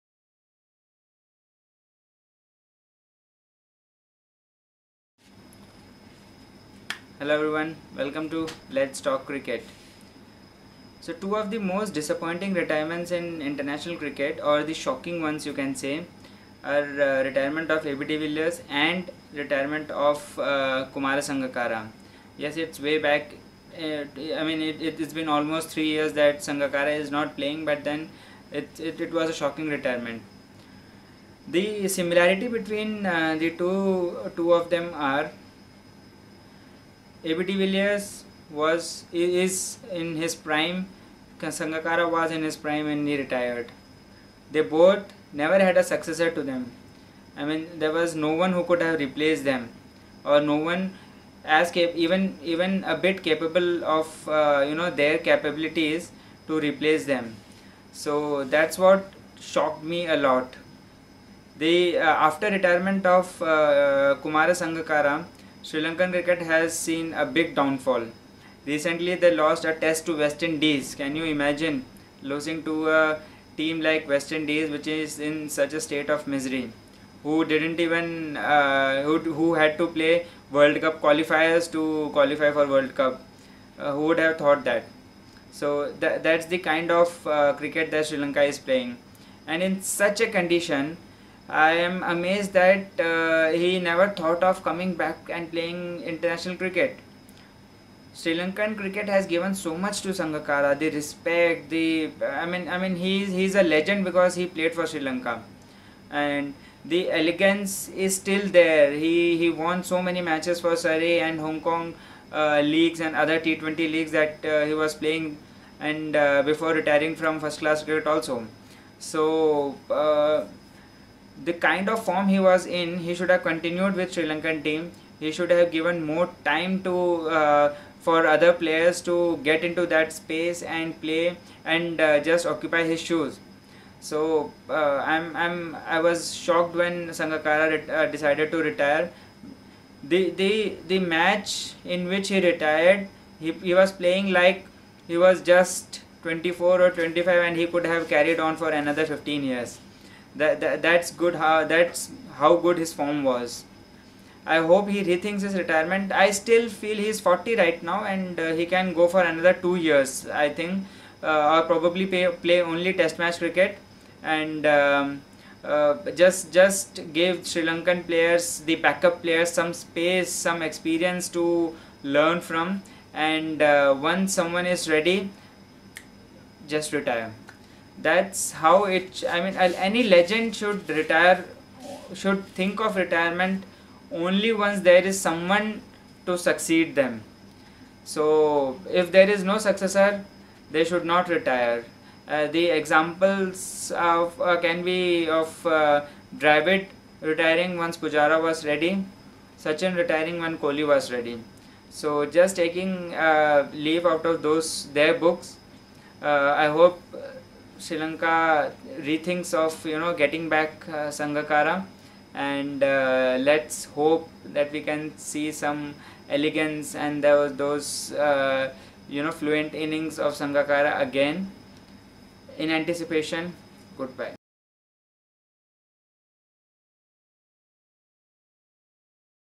Sangakara's Retirement was also Disapointing
Since Sangakara has retired Sri Lankan Cricket has seen a big downfall...